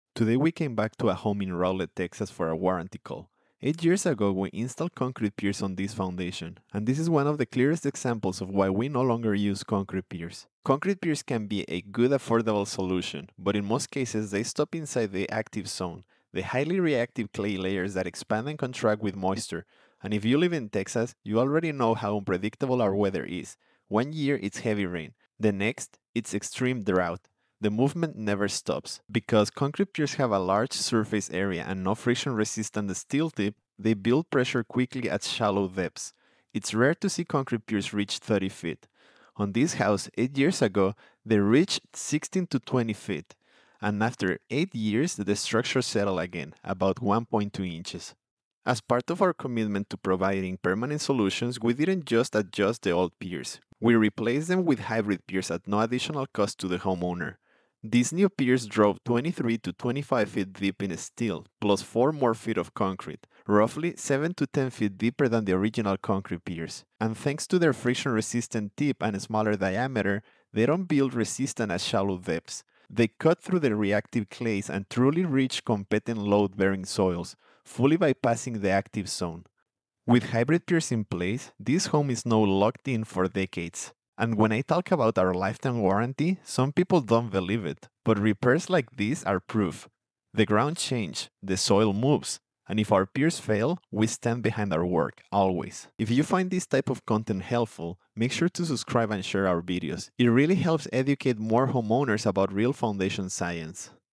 Why We Stopped Using Concrete Piers | Real Warranty Case in Rowlett, Texas
Eight years ago we installed concrete piers on this home in Rowlett, Texas. Today, after the foundation settled again, we returned under our lifetime warranty. This case shows exactly why we transitioned to hybrid piers: concrete piers often stop inside the active zone, within highly reactive clays that expand and contract with every season in Texas.

Our hybrid piers drove 23–25 feet deep in steel plus 4 more feet of concrete, reaching true load-bearing strata and providing a long-term, reliable solution at no cost to the homeowner.

It helps more homeowners protect their homes and avoid costly mistakes.